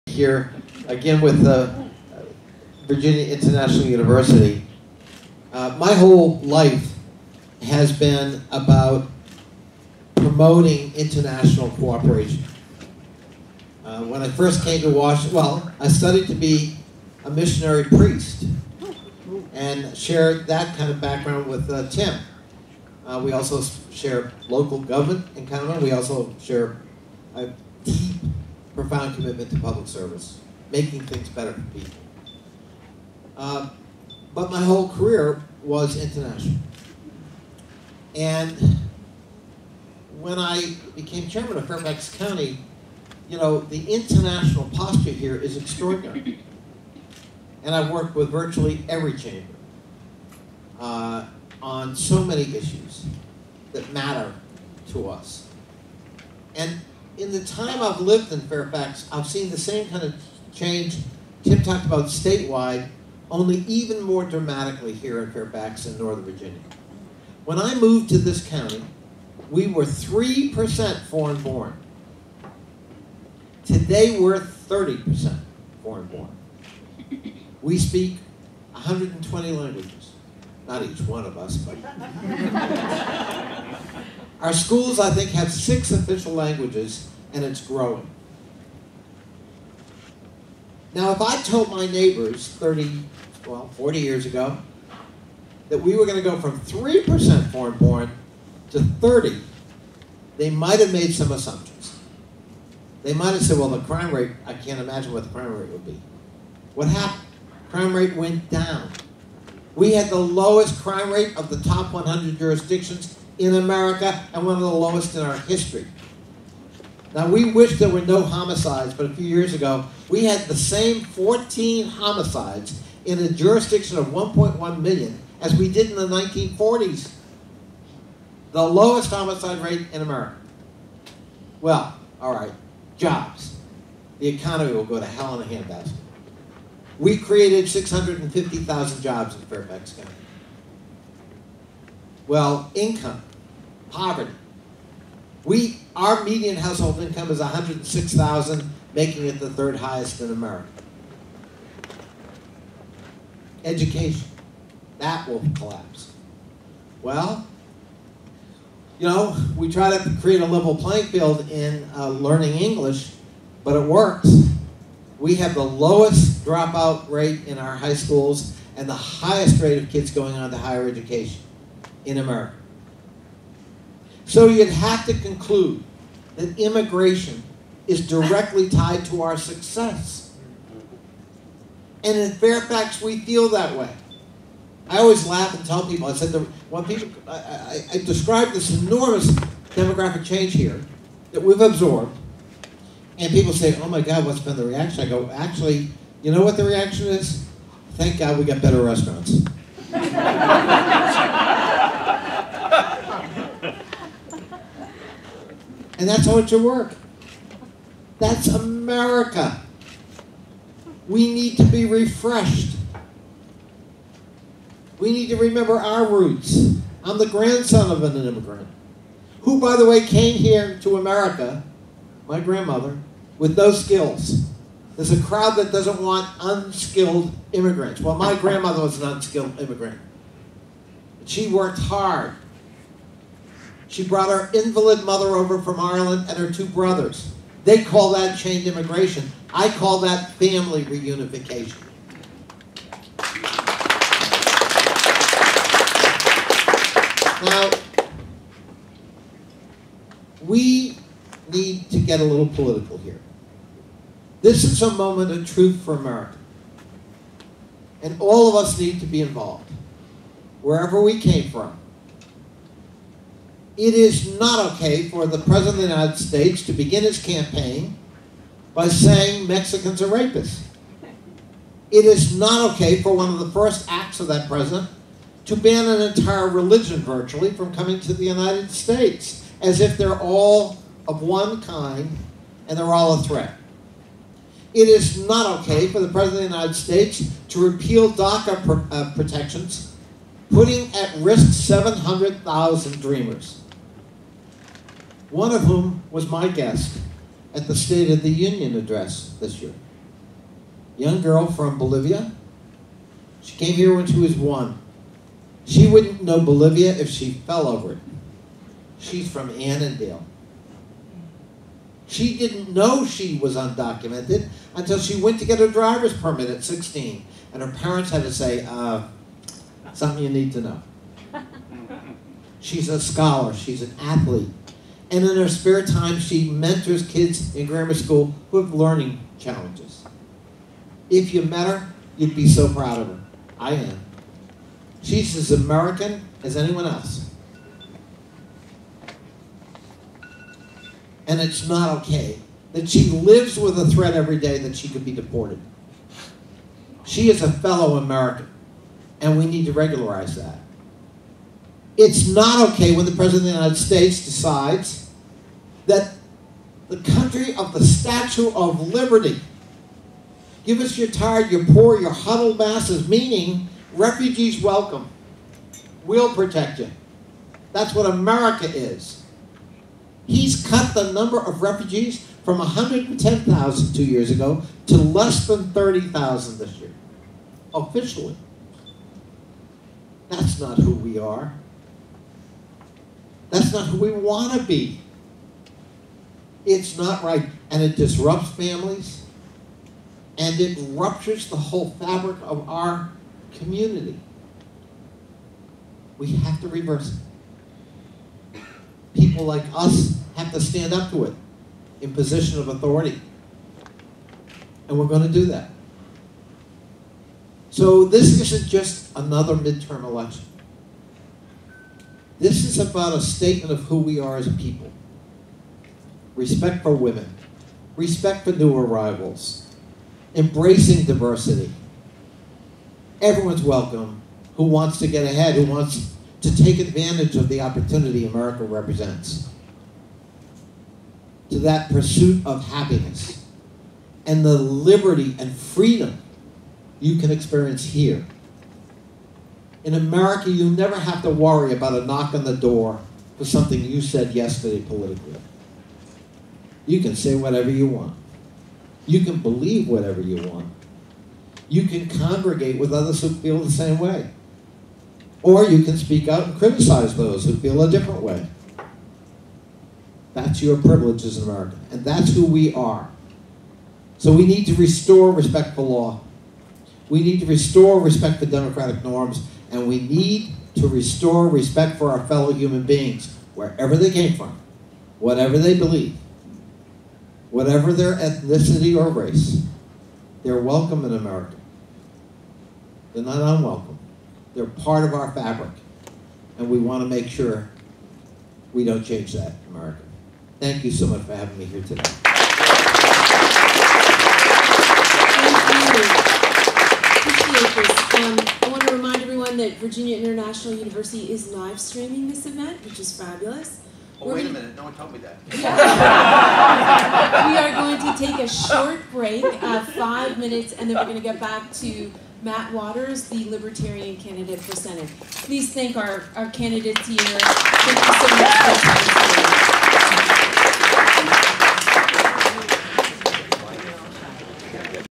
Candidates Forum 2018 in Northern Virginia - Democratic Candidates - Raw File 3
Candidates Forum 2018 in Northern Virginia by The Multicultural Chambers Alliance at Virginia International University on Sunday, September 30, 2018.
Congressman Gerald E. Connolly, Virginia's 11 District